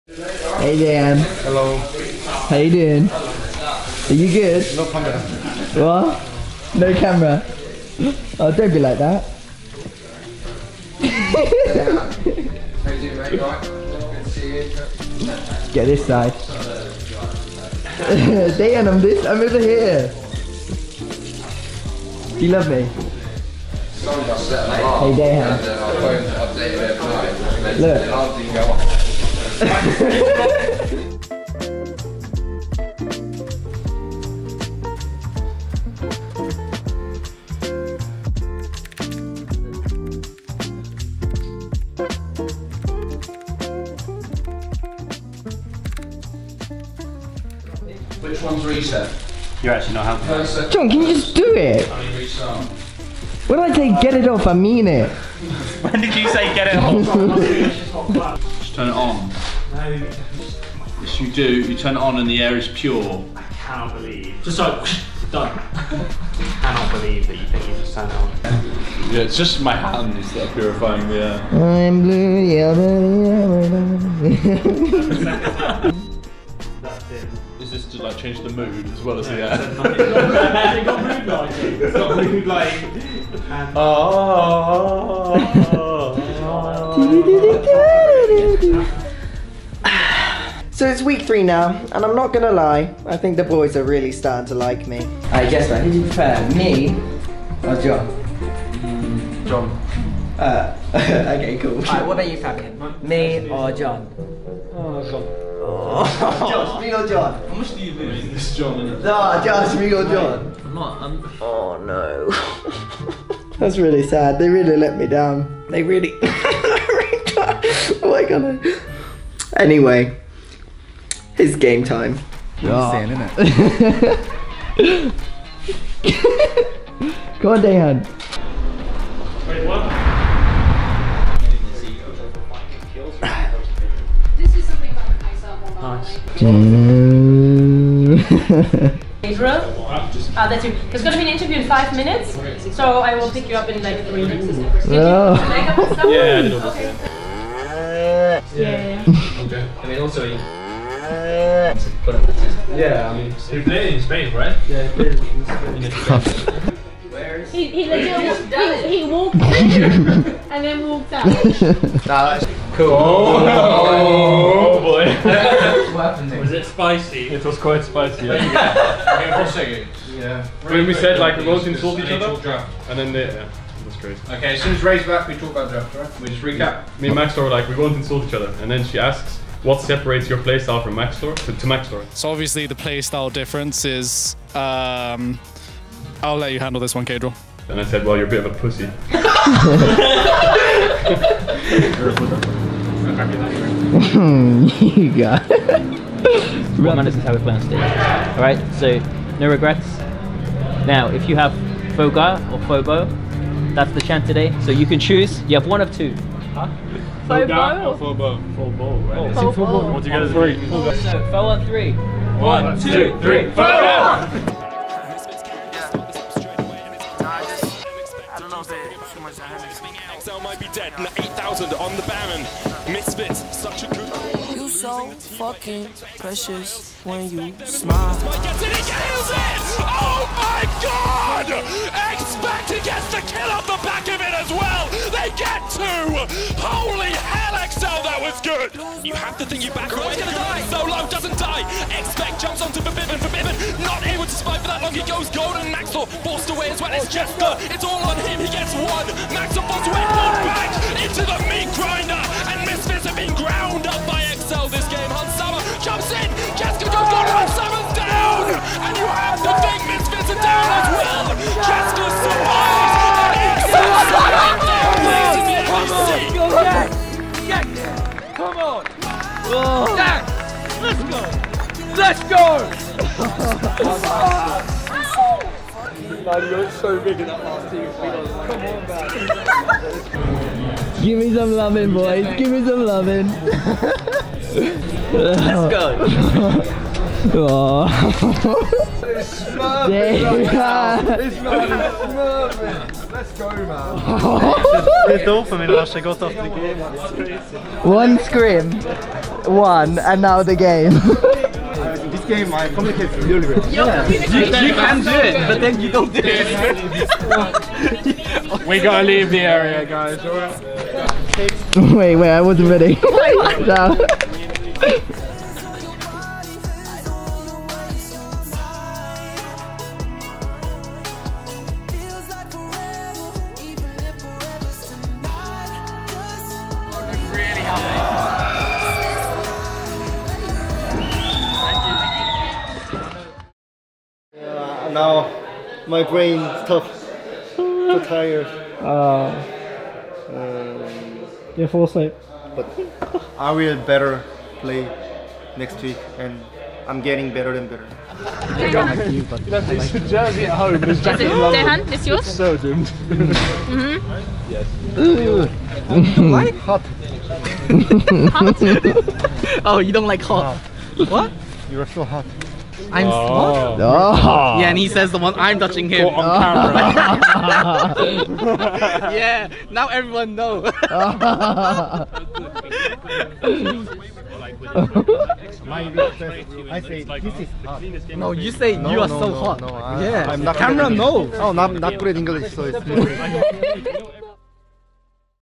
When Baehan Smites | The LEC Vlog | Episode 5 vs. Misfits Gaming
Watch the fifth episode of The Excel LEC Vlog from our game against Misfits Gaming. When Baehan Smites 😍

Why not visit our headline sponsor OMEN by HP whilst you are at it